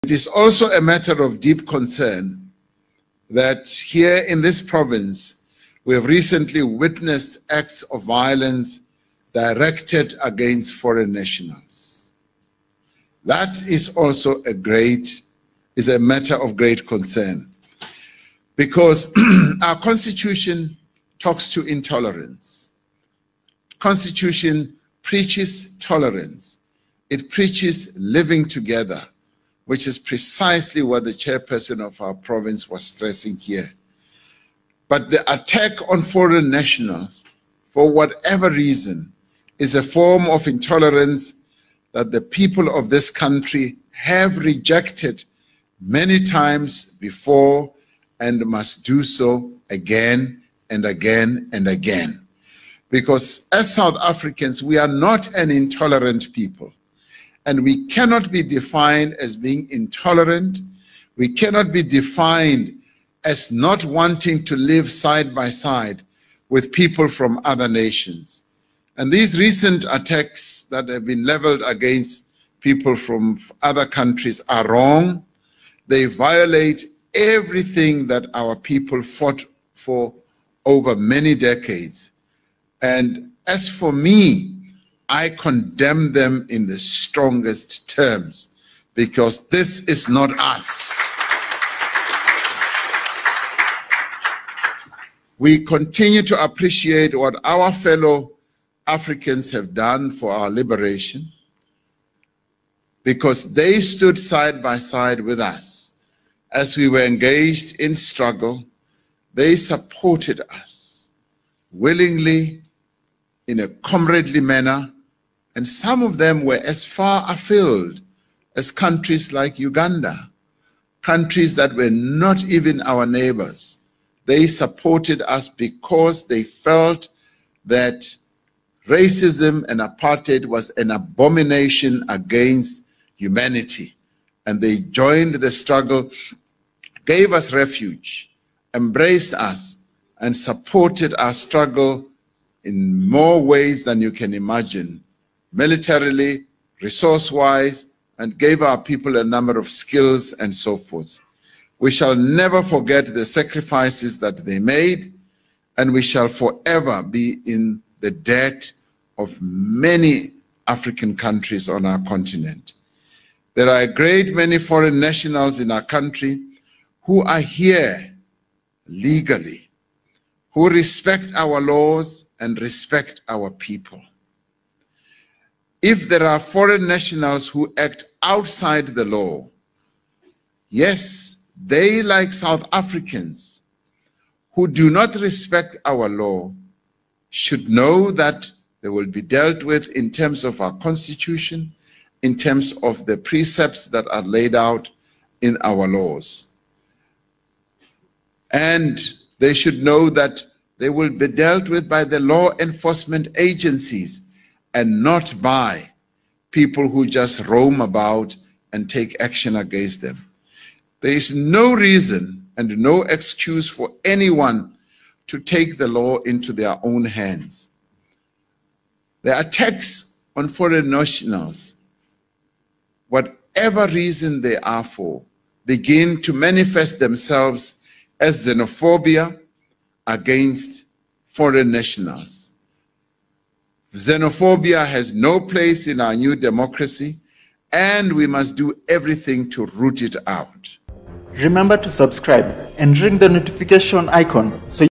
President speech after the xenophobic attacks in South Africa 2019
The president talk about the afrophobia / xenophobia in south africa 2019 and how african nations helped the country in apartheid time